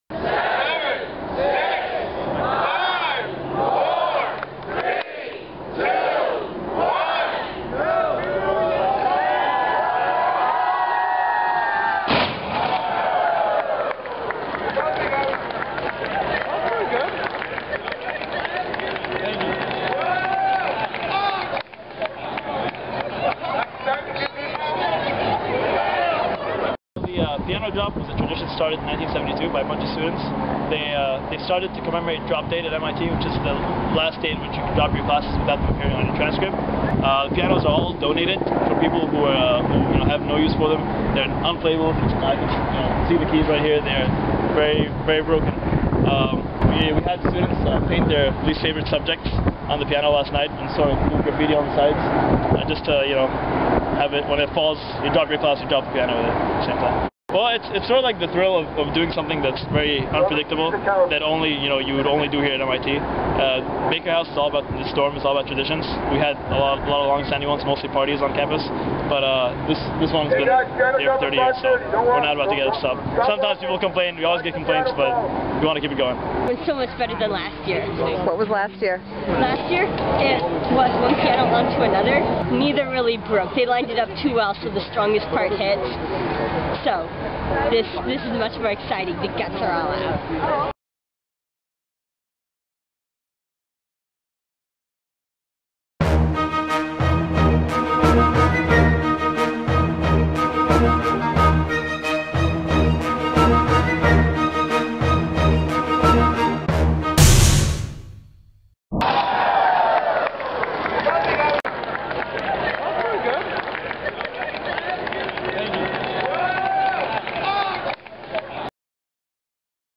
MIT's annual Piano Drop in Cambridge - 2009
MIT honors a tradition dating back to 1972 by hurling a 500-lb piano off the roof of the Baker House dorm on Memorial Drive on Thursday, April 23, 2009.